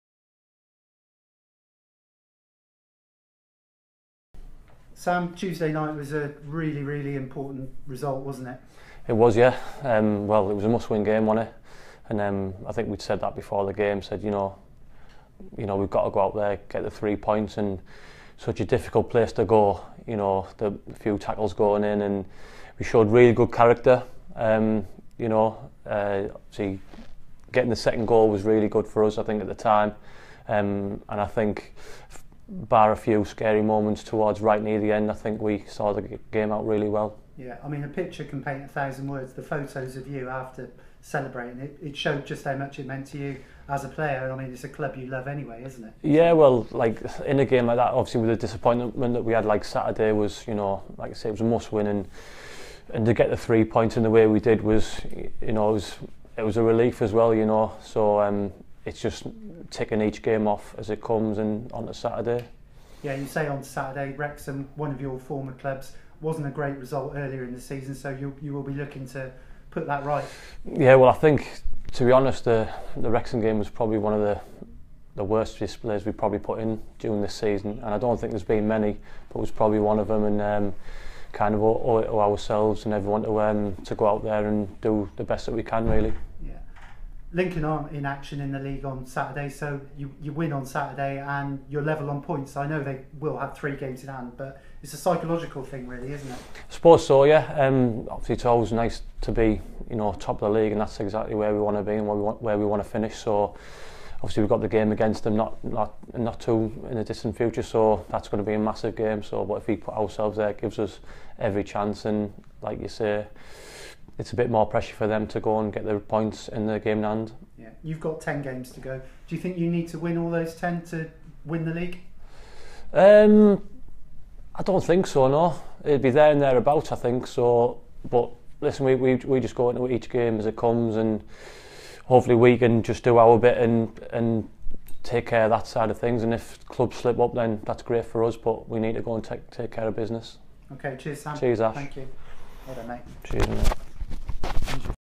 Sam Russell
Forest Green goalkeeper Sam Russell is taking each game as it comes.